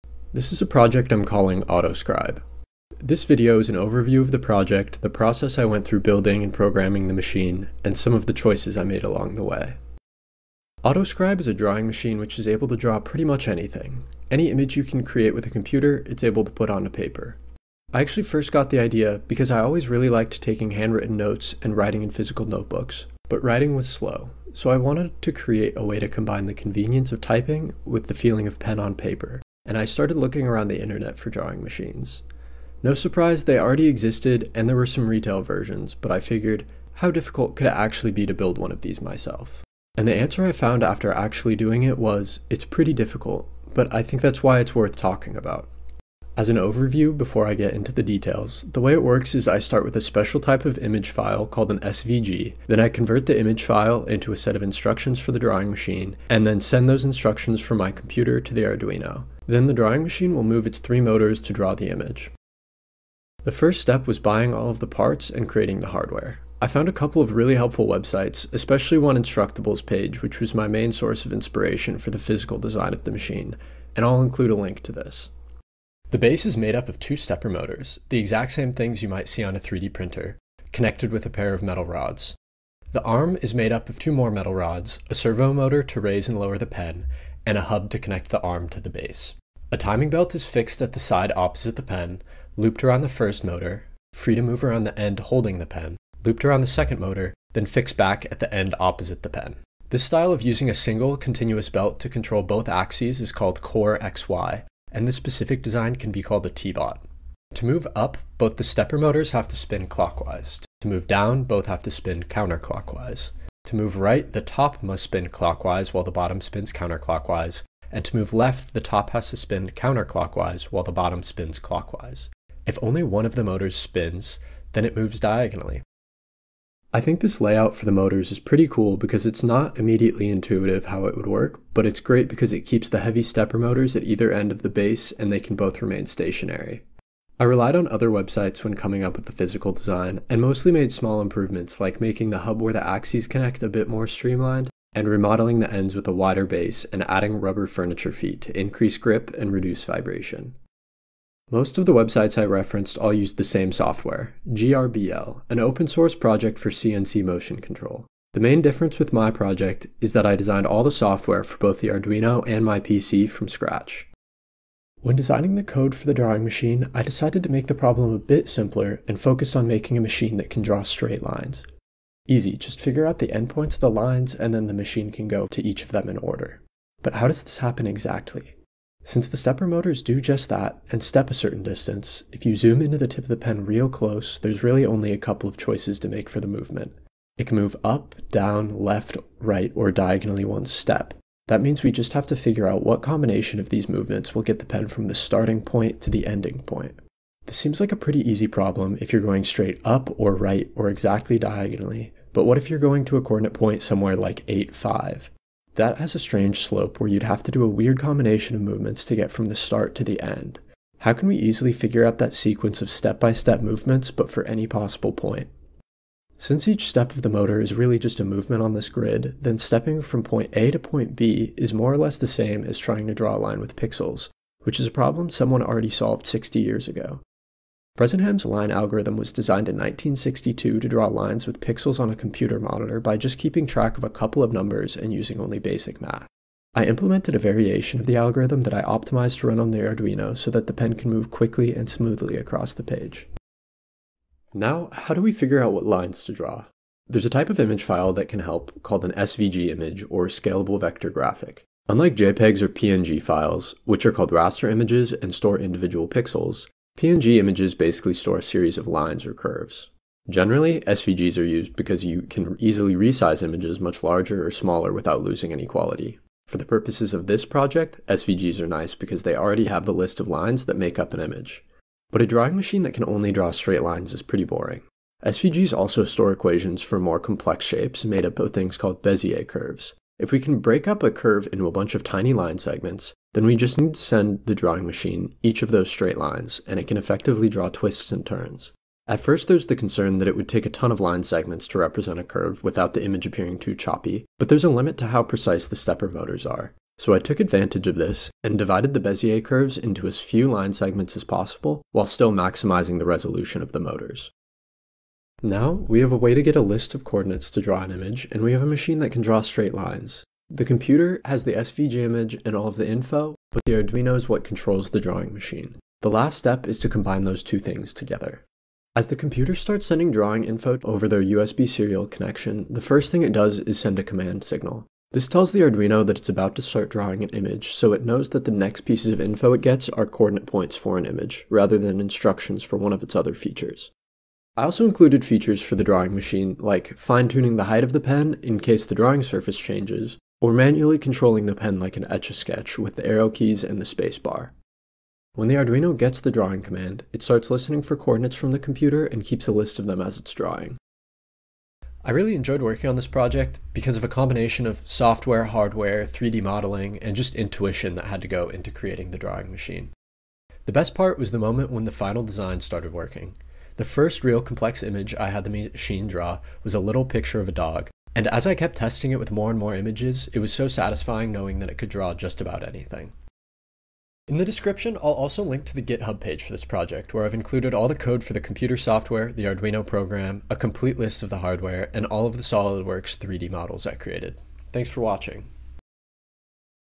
AutoScribe - CNC Drawing Machine
AutoScribe is a drawing machine capable of making any piece of art that can be drawn with pen on paper. On a computer, it converts SVG images into a set of points which are sent to the Arduino and drawn.

0:00 Intro
1:15 Hardware
2:30 Software
2:44 Arduino Code
4:07 SVG files
5:04 Putting it together
5:51 Conclusion